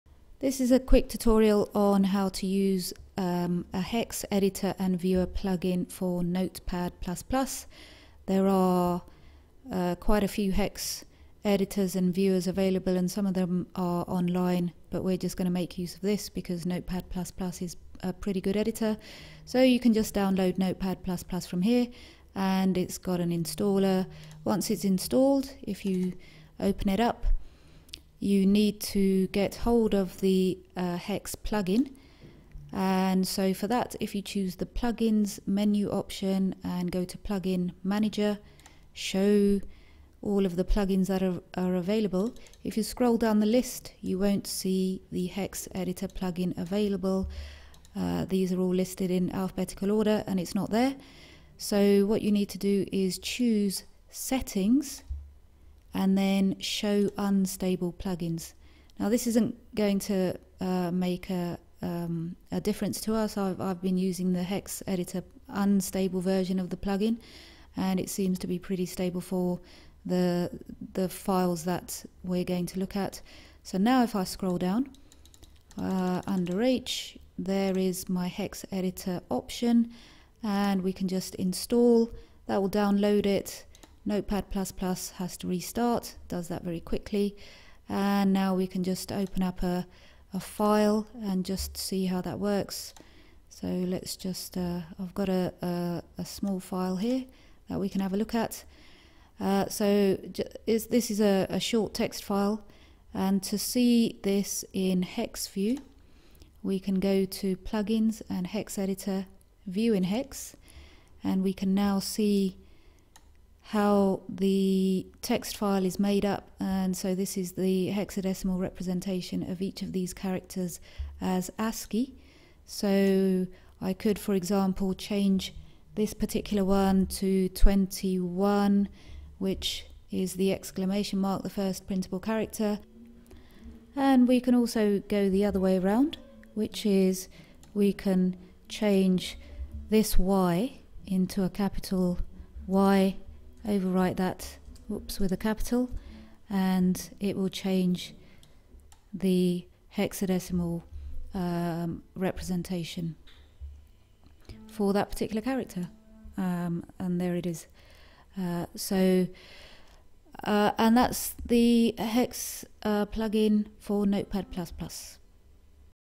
Notepad++HEXeditor
How to use the hex editor plugin for notepad++